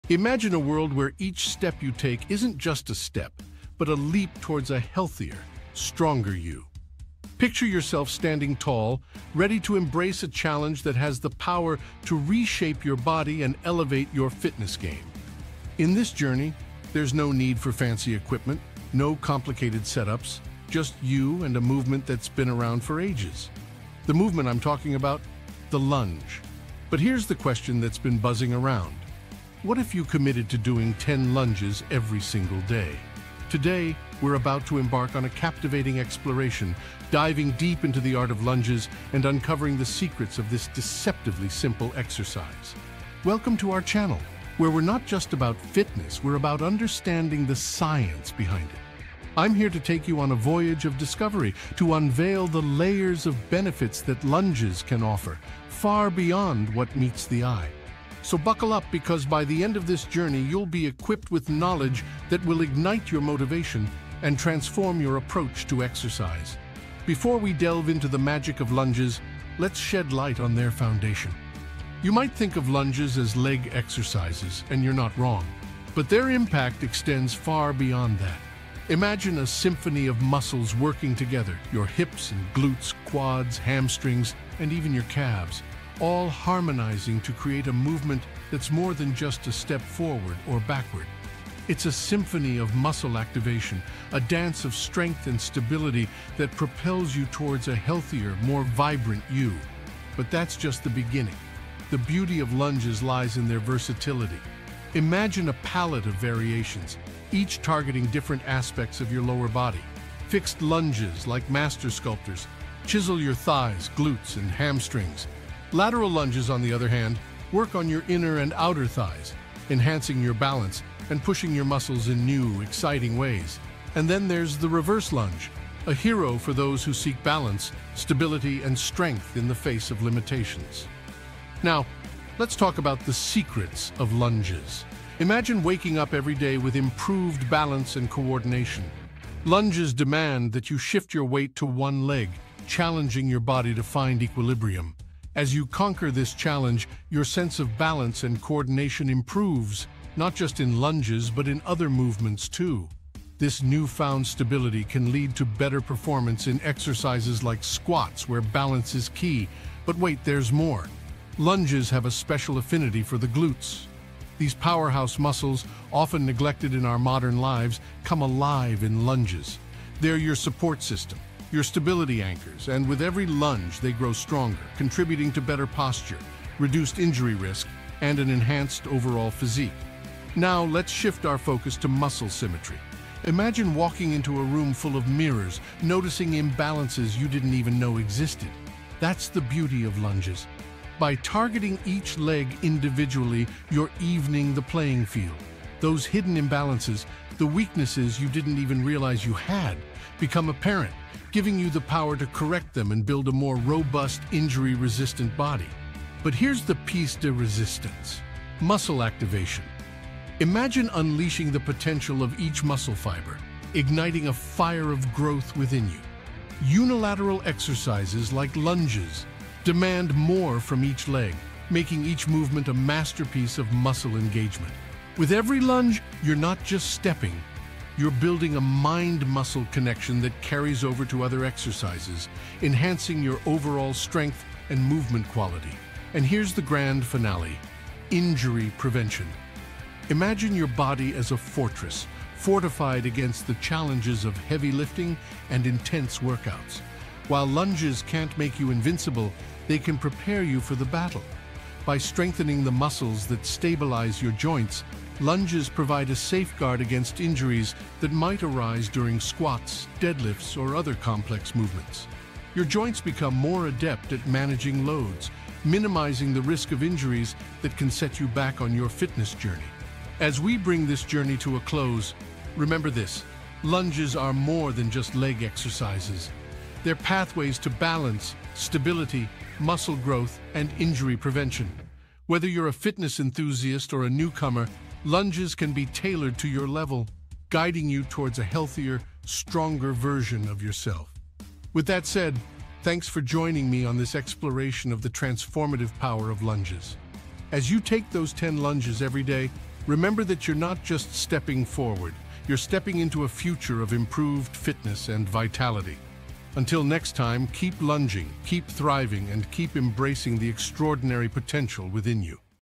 What Happens To Your Body When You Do 10 Leg Lunges A Day!
🔥 Ready to sculpt those legs and build lower body strength like never before? Join us in this exciting 10-day leg lunge challenge! Whether you're a fitness enthusiast or just starting your fitness journey, these 10 leg lunges a day will make a noticeable difference in your leg muscles and overall endurance.

🏋️‍♀️ Challenge yourself to incorporate 10 leg lunges into your daily routine and watch your legs transform. This simple yet effective exercise targets your quads, hamstrings, glutes, and calves, helping you achieve toned and strong legs.

🎯 Here's what you can expect from this challenge:
1️⃣ Improved Leg Strength
2️⃣ Enhanced Muscle Definition
3️⃣ Increased Balance and Stability
4️⃣ Enhanced Endurance
5️⃣ Better Posture

💪 Ready to get started? Make sure to track your progress and share your journey with us in the comments section below.

📆 Schedule:
Day 1-10: Perform 10 Leg Lunges (each leg) daily.
Optional: Add weights for an extra challenge!

Remember, consistency is key to achieving your fitness goals.

🎉 Let's embark on this leg-tastic journey and transform our lower bodies one lunge at a time! Get ready to level up your leg game! 💥